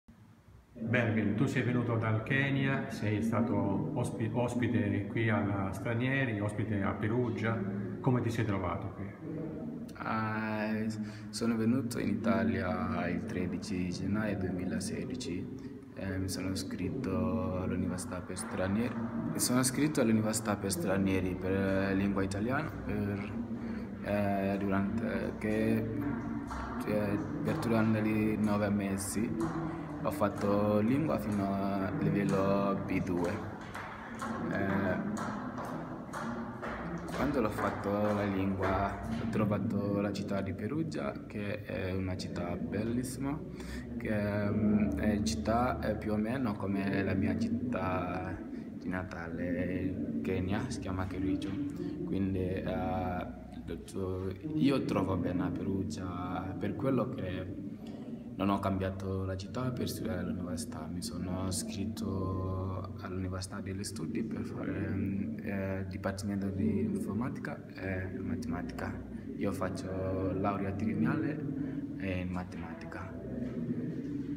Intervista a Cheruiyot Bervin, lo studente di Garissa ospitato dalla Stranieri
Presentato in anteprima alla Stranieri “Buio a Mezzogiorno, Al Shabaab nel nord est del Kenya”, il film documentario di Angelo Loy e Tommy Simmons - Sala Goldoni di Palazzo Gallenga – Università Per Stranieri di Perugia. Erano presenti all’evento il Magnifico Rettore, prof. Giovanni Paciullo, la docente di Storia e Istituzioni dell’Africa dell’Università per Stranieri di Perugia Federica Guazzini, insieme, tra gli altri, ad alcuni studenti keniani scampati alla strage di Garissa del 2015  e ospitati dalla Stranieri con un soggiorno di studi.